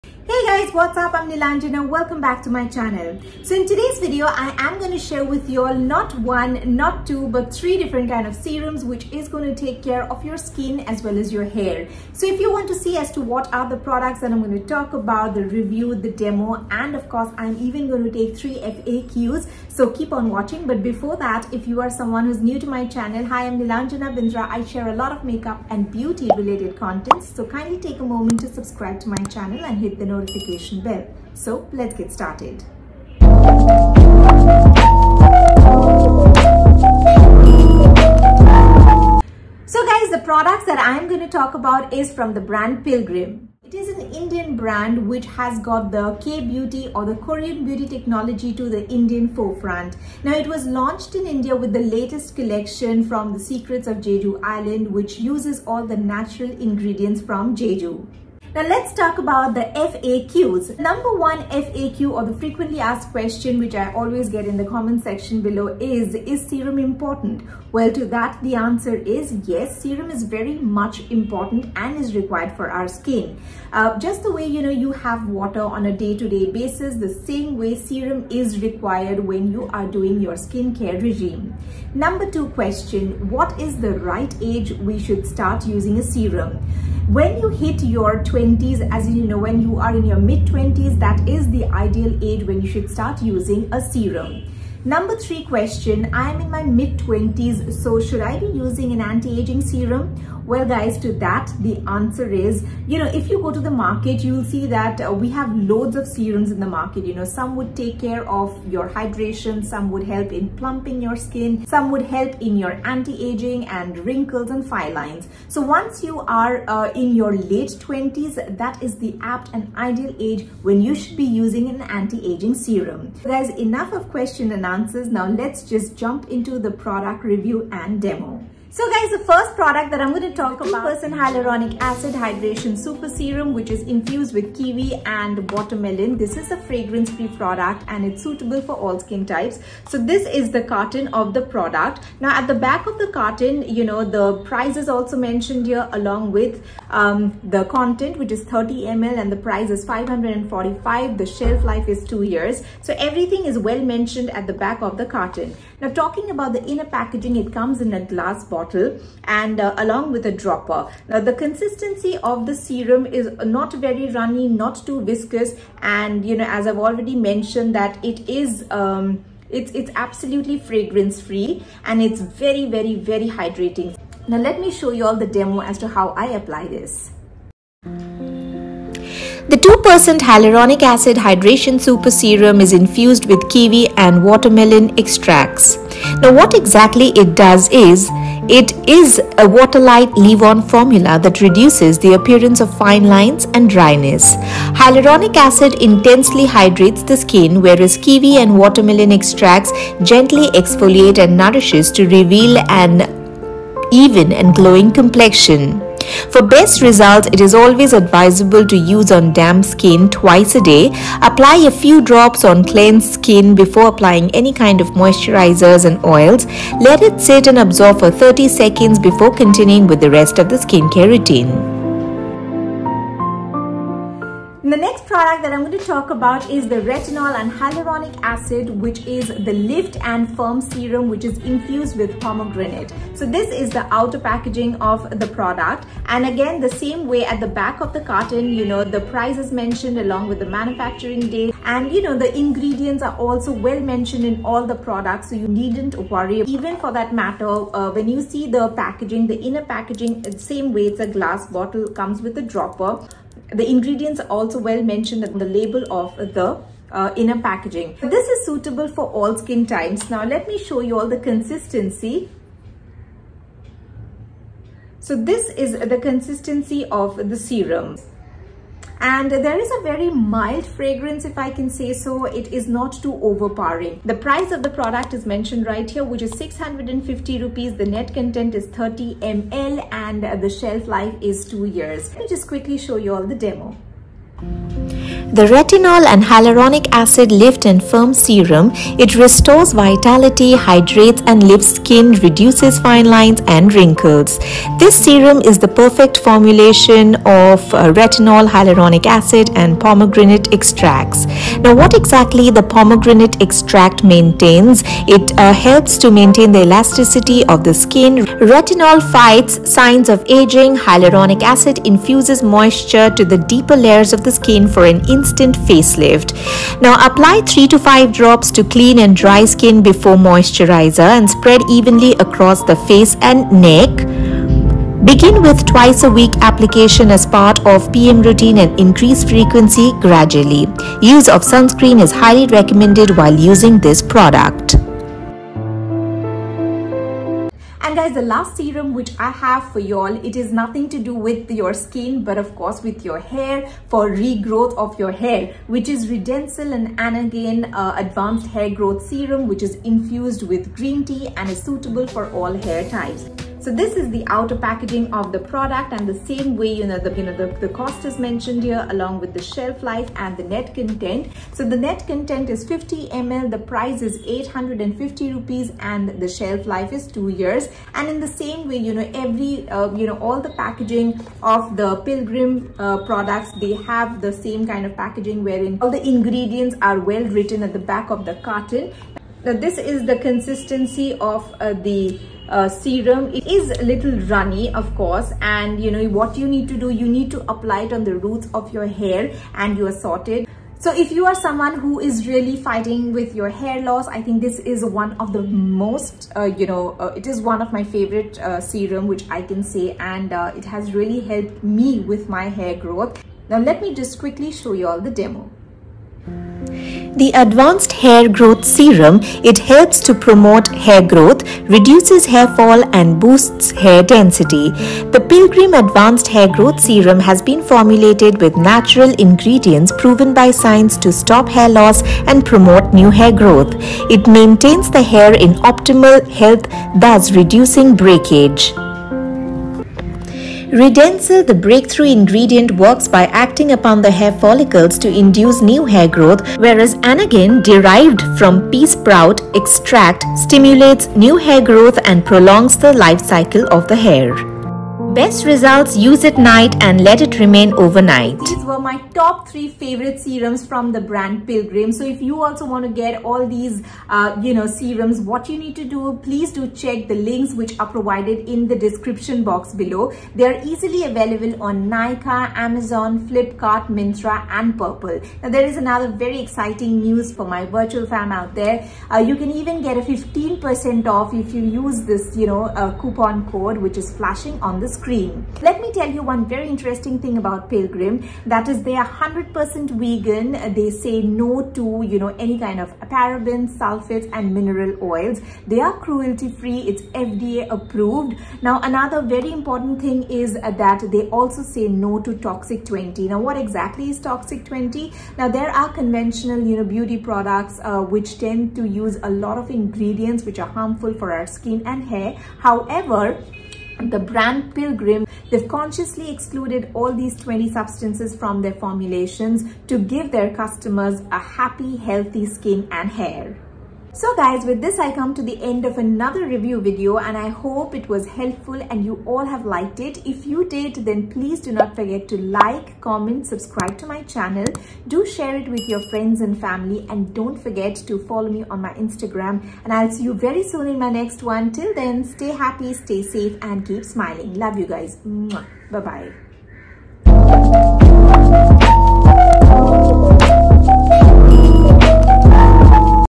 My Top 3 Favourite Serums From Pilgrim
Serums does wonders to our skin & hair. Here’s  listing down below my 3 Most Trusted & Favourite Serums From Pilgrim .

- 2% HYALURONIC ACID HYDRATION SUPER SERUM
- ADVANCED HAIR GROWTH SERUM
- LIFT AND FIRM SERUM